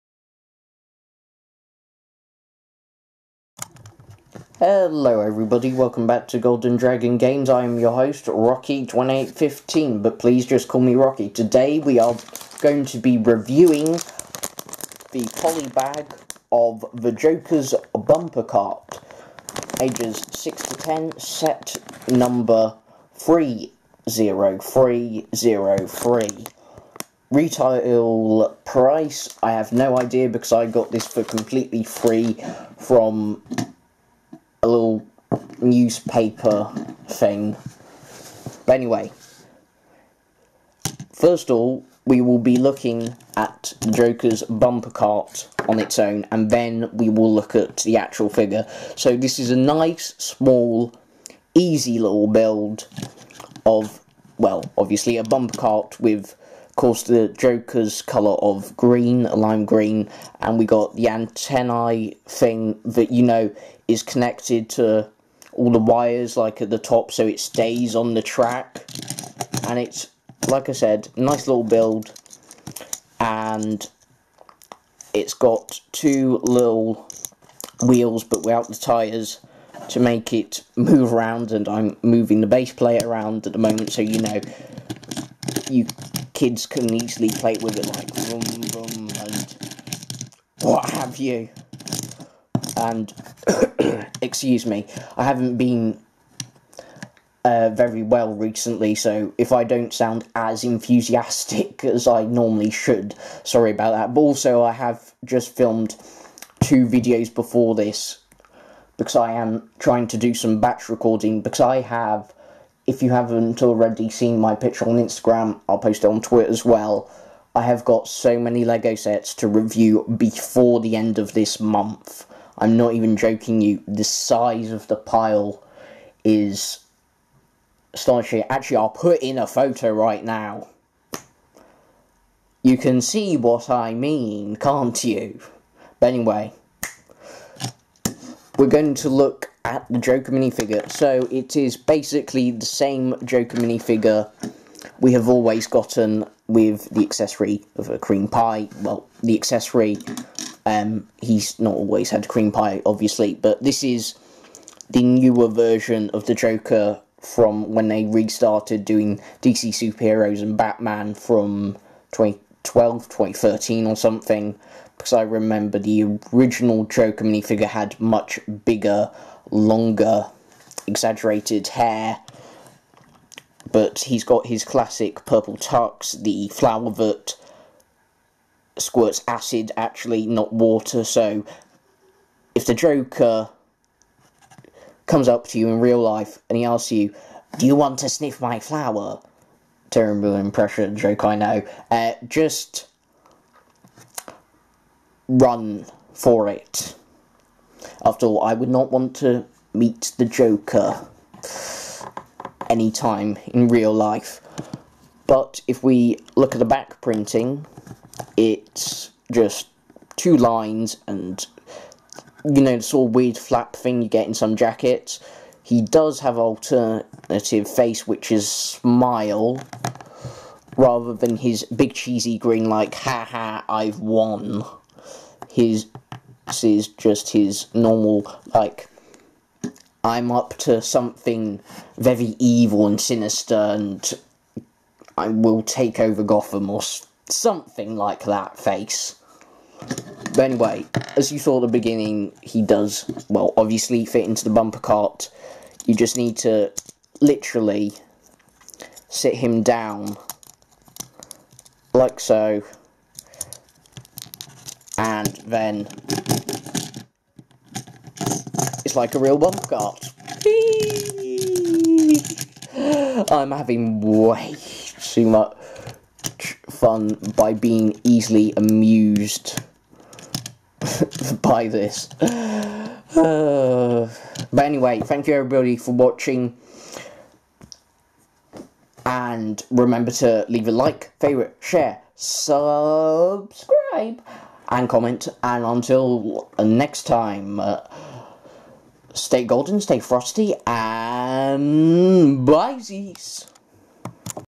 Lego Joker Bumper Car Polibag Reivew
hello everybody Rocky back with another lego review to day we have the lego dc super heroes Joker's bumper car polibag hope you all enjoy the video and remember to leave a like,fav,share,comment and subscribe and until next time stay golden stay frosty and BBBBYYYYEEEEZZZZSSSS